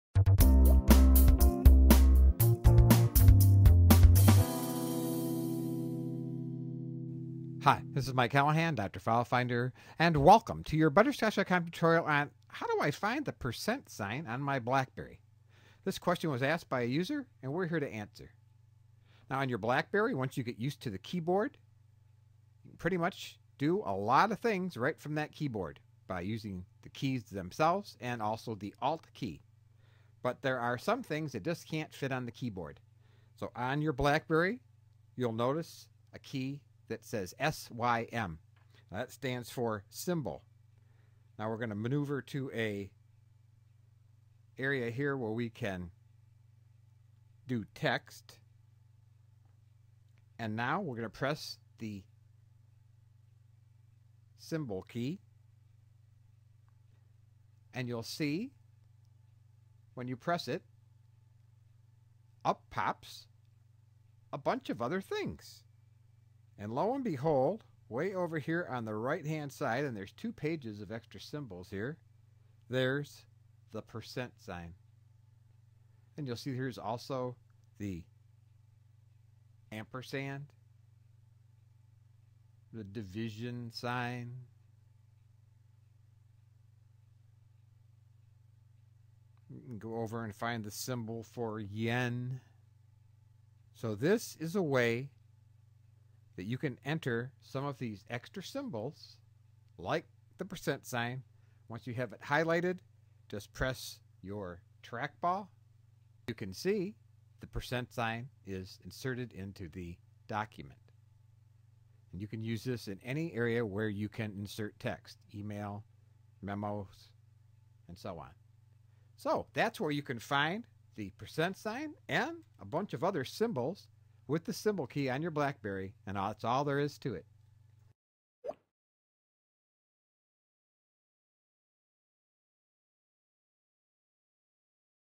Finding hidden symbols on BlackBerry devices
Some characters on your BlackBerry aren't on the main keyboard. Here's how to find them. A Internet tutorial by butterscotch.com.